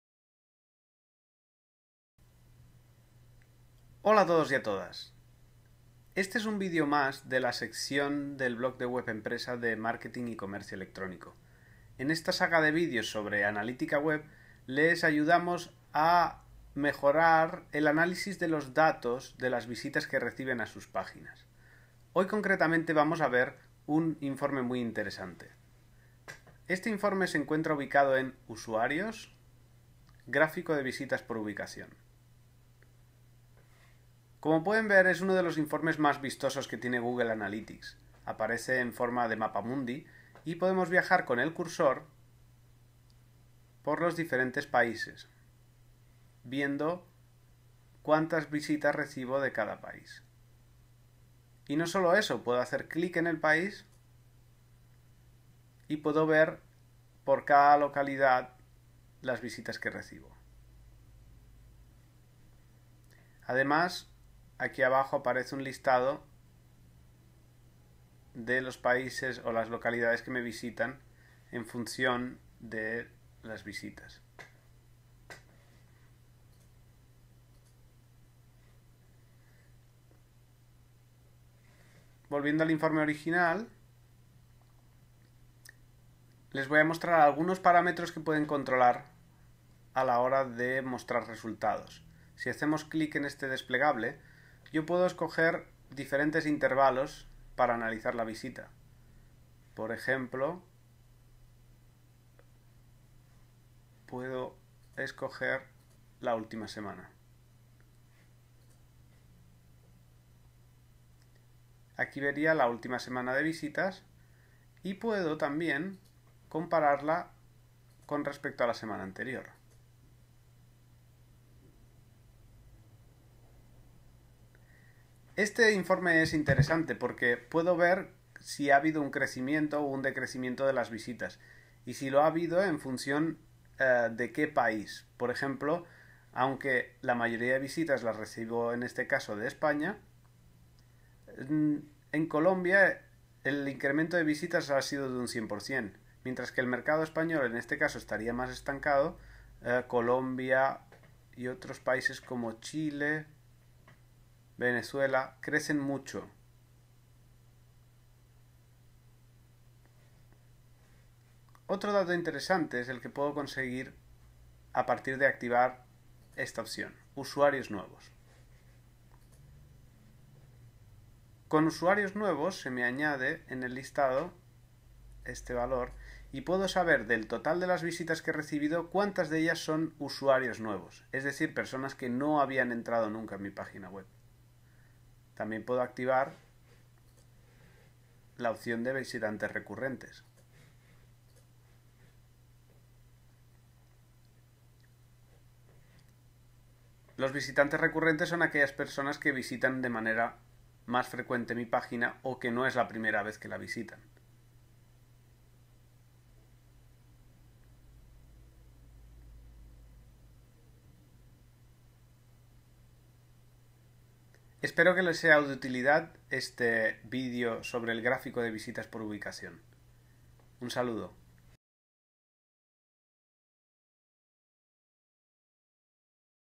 Informe de visitas por países en Google Analytics
En este artículo hablaremos del "gráfico de visitas por ubicación" un informe que aparece en las estadísticas sobre visitas en su web en Google Analytics. Si aún no ha configurado Analytics puede encontrar más información en el siguiente enlace

El "gráfico de visitas por ubicación" es un informe que muestra las visitas a su página web en función del lugar de procedencia. Es una función muy interesante para evaluar nuevos mercados.